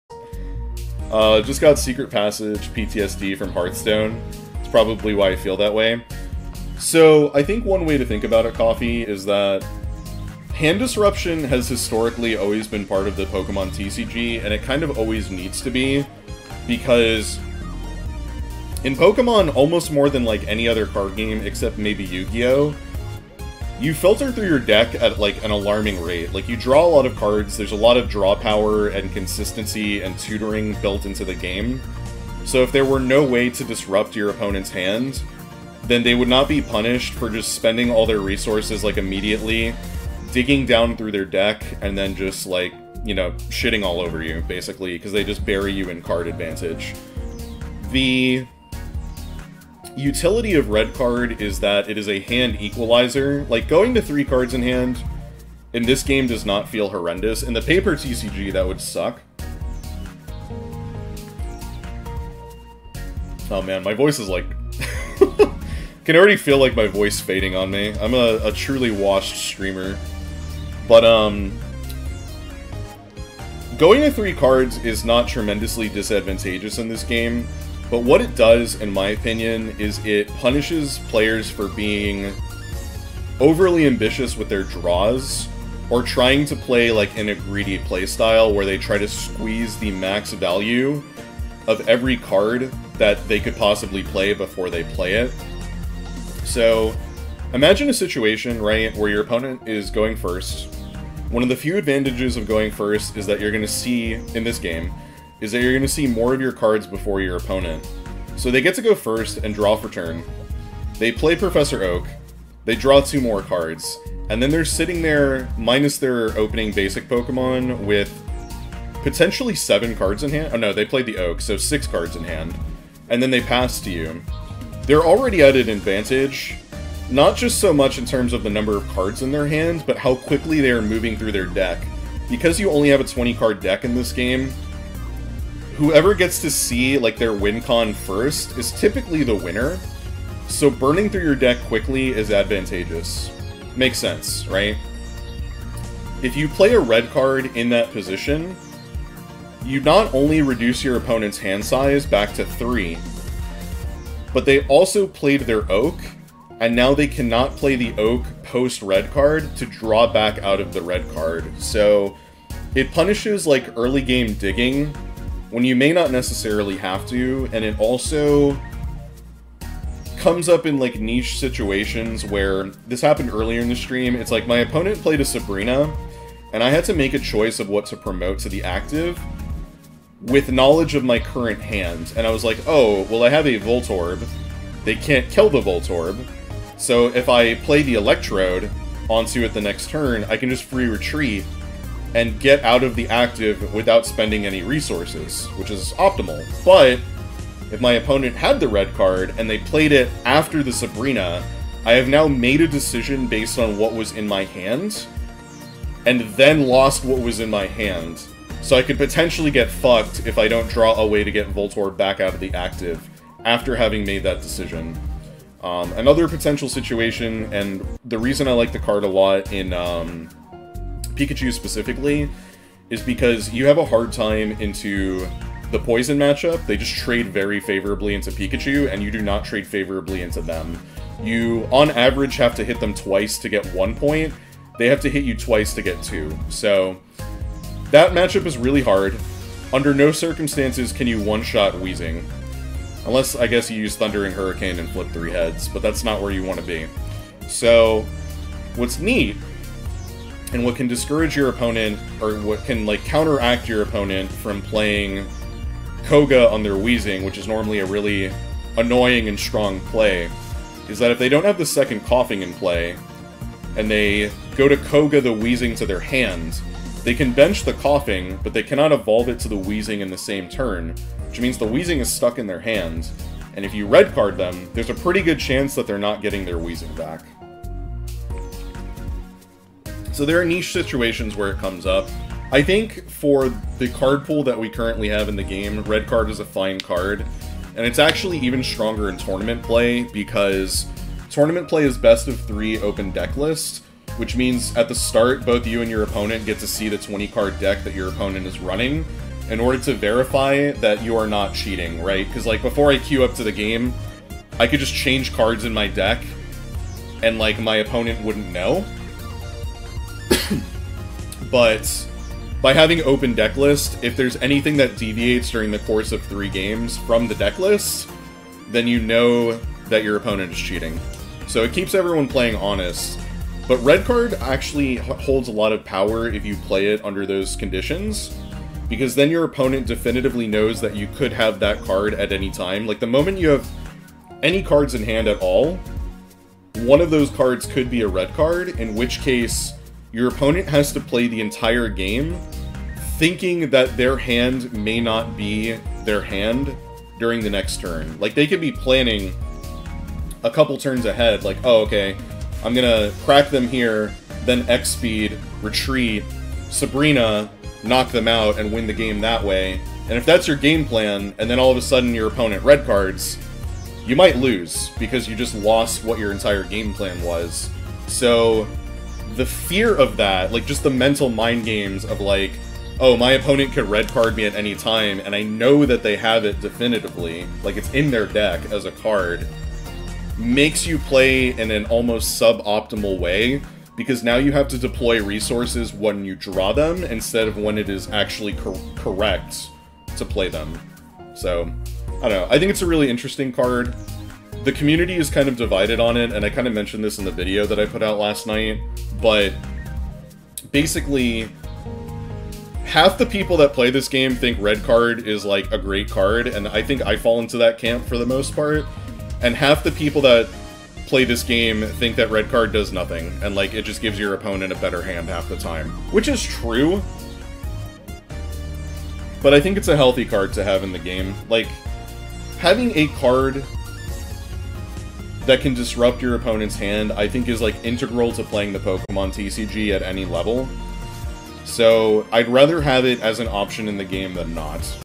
Pokémon TCG Pocket - Is Red Card Healthy for the Game?
A discussion which took place during my last live stream.